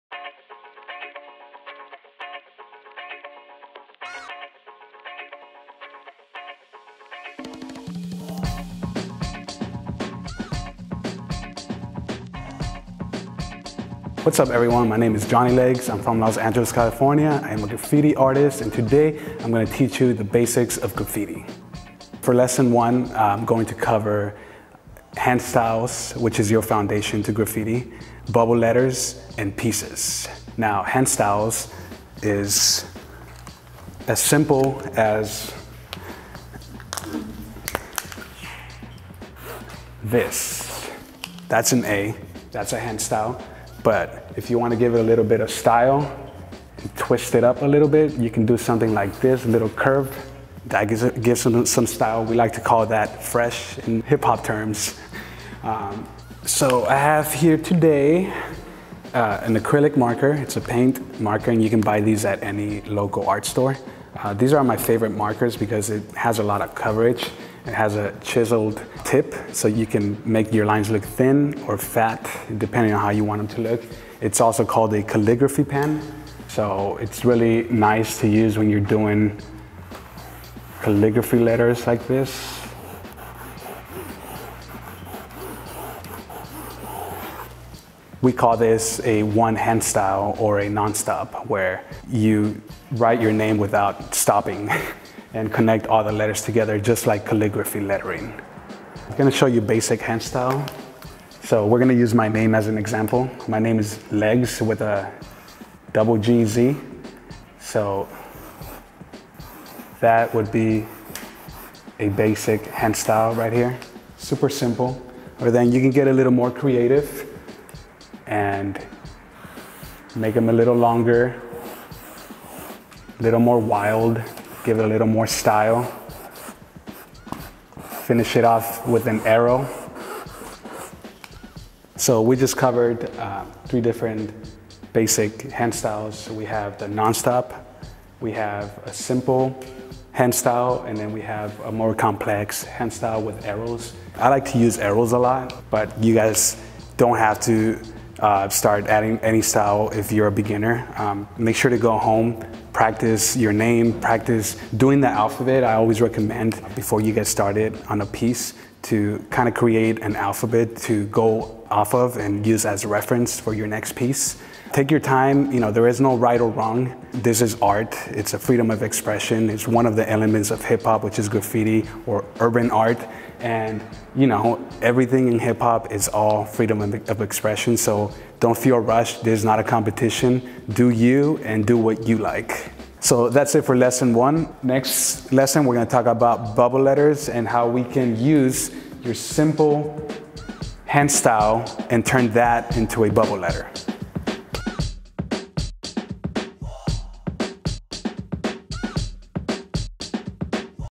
Urban Art English Lesson 1 with Johnny Leggz | JUICE
JUICE presents our Hip Hop Arts Tutorial Series with our first lesson on Urban Art in English with Johnny Leggz

Remember to watch the next lesson until the end, where we put all the moves together for a final showcase!

In collaboration with California Arts Council